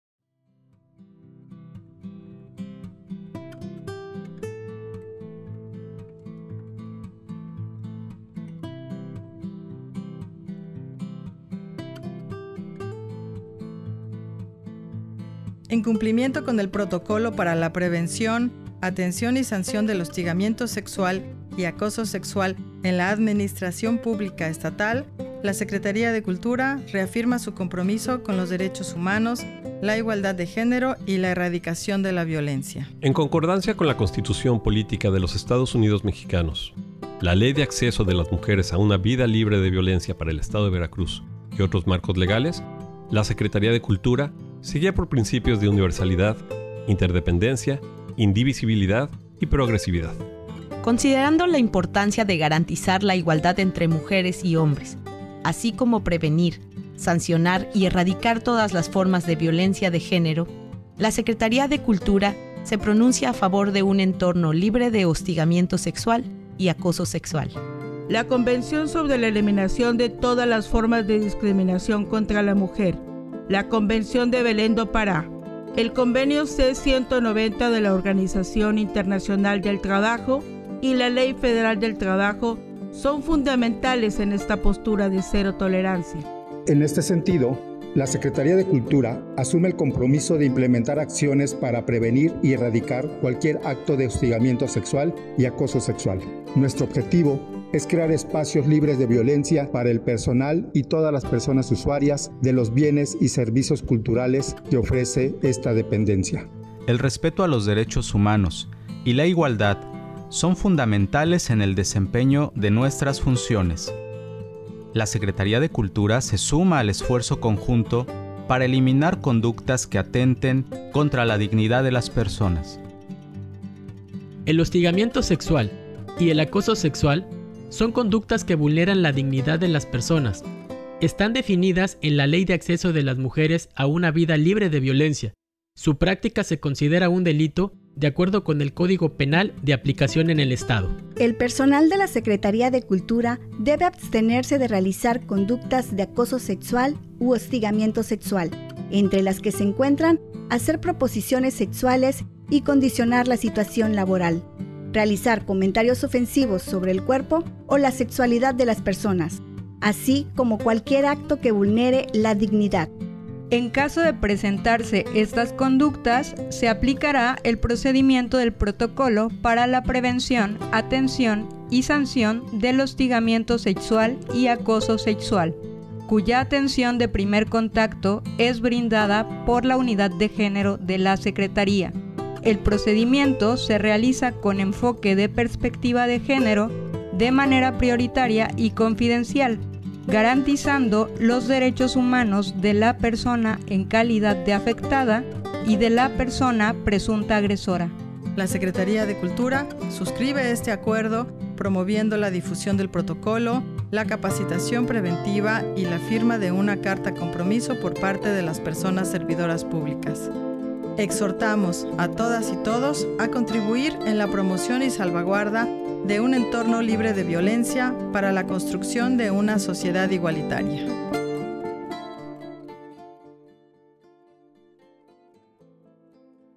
Pronunciamiento Cero Tolerancia
Este 8M, en la SECVER reafirmamos nuestro compromiso con los derechos humanos, la igualdad de género y la erradicación de la violencia.

Exhortamos a todas y todos a contribuir en la promoción y salvaguarda de un entorno libre de violencia para la construcción de una sociedad igualitaria.

Conoce el Pronunciamiento cero tolerancia al hostigamiento sexual y al acoso sexual

Este programa es público y ajeno a cualquier partido político. Queda prohibido su uso con fines distintos a los establecidos en el programa.